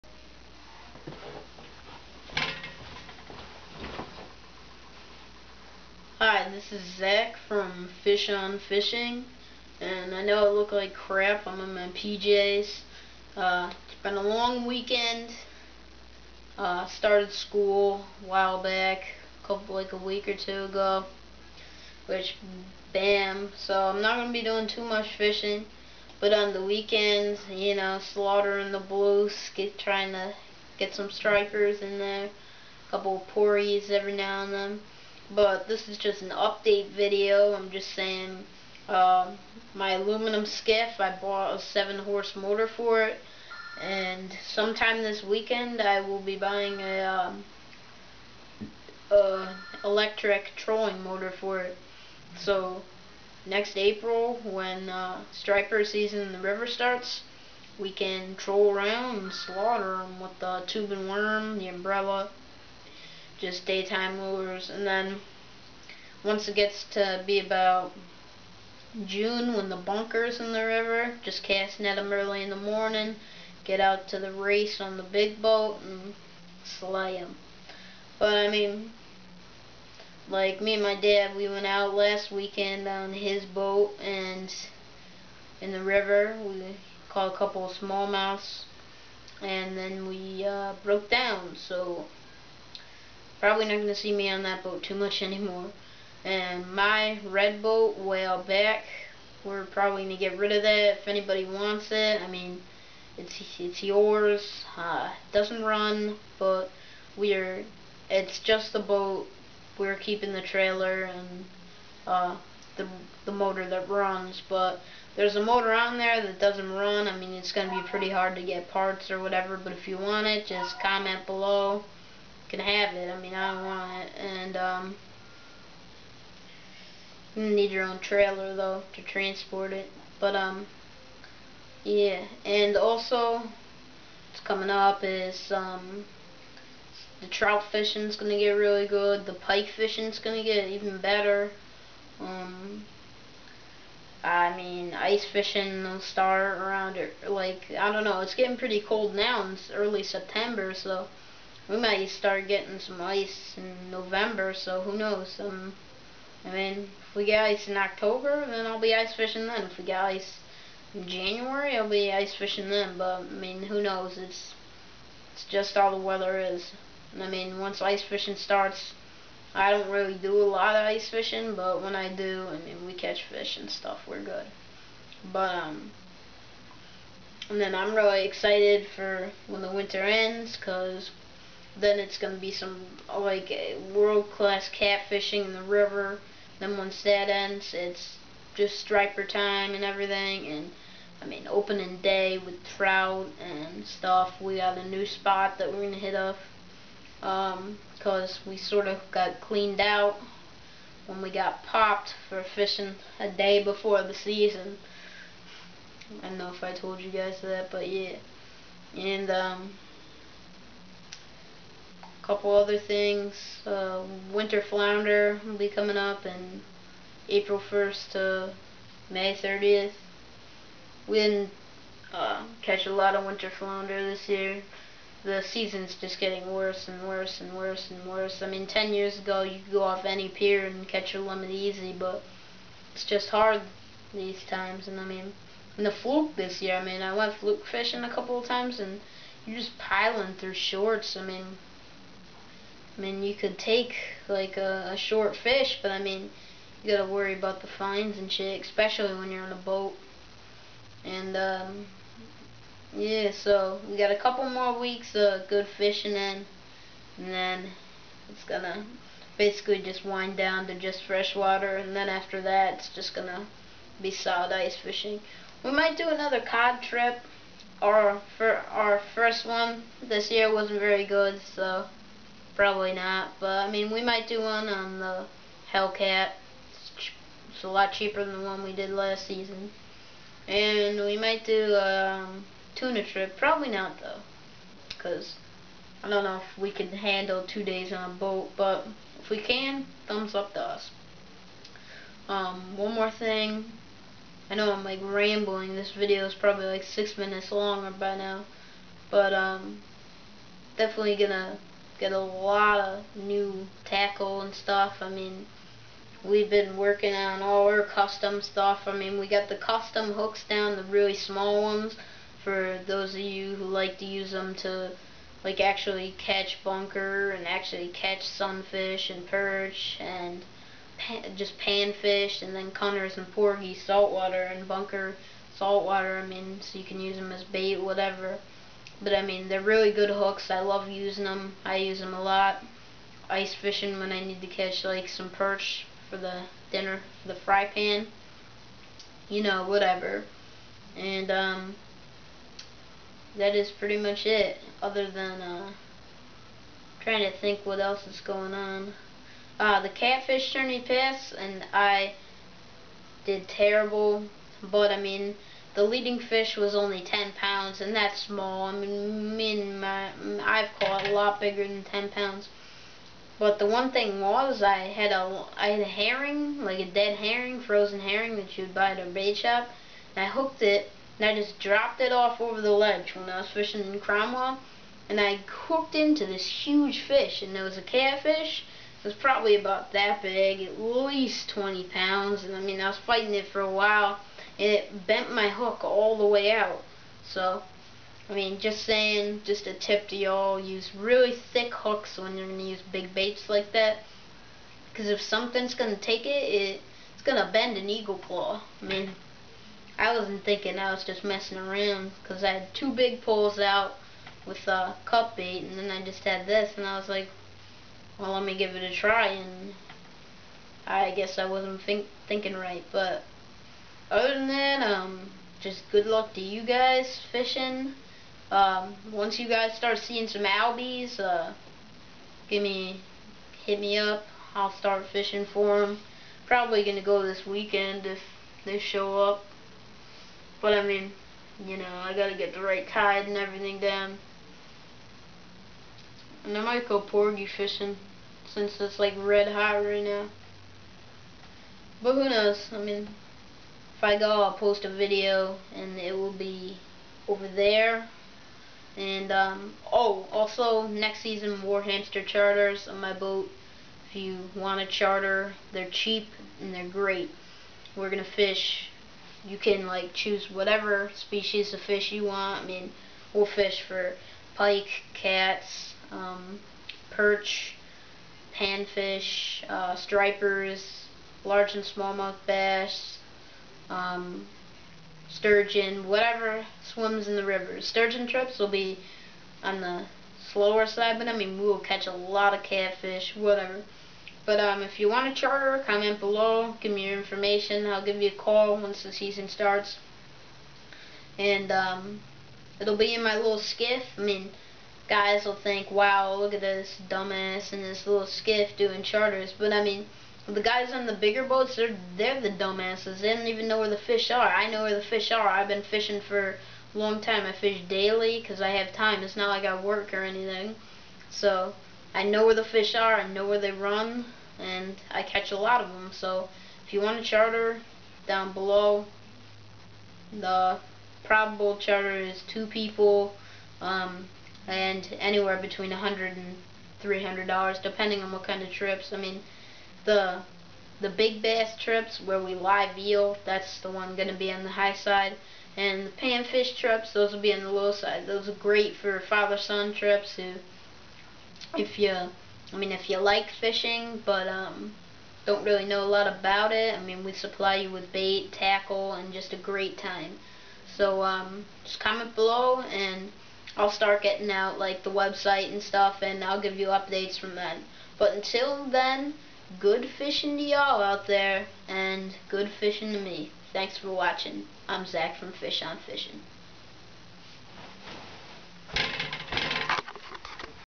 Update Video
updates on trout, pike, albies, and porgies. be sure to book a spot on warhamster 74's boat charters next season!